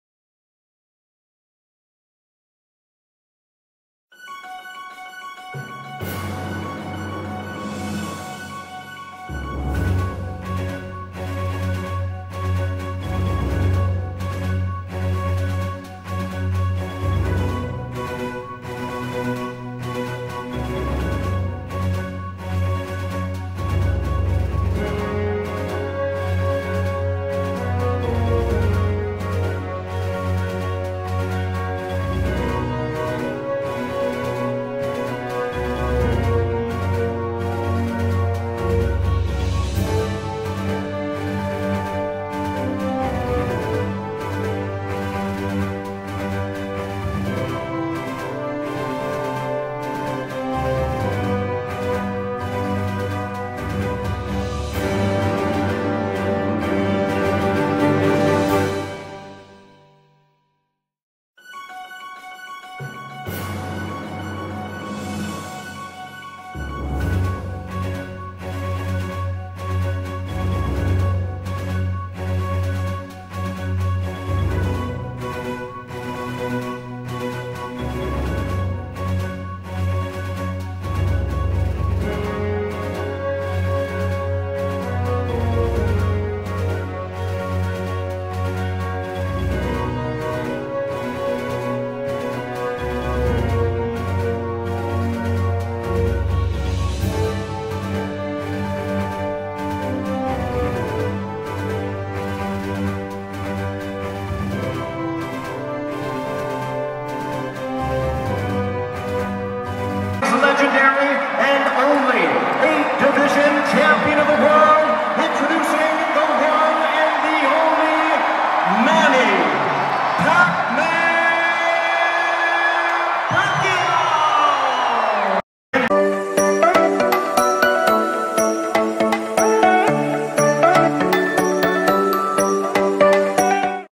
TWITTER (CELEBRITIES, ATHLETES) REACT TO PACQUIAO UGAS POST FIGHT
twitter world erupts after the upset of UGAS PACQUIAO fight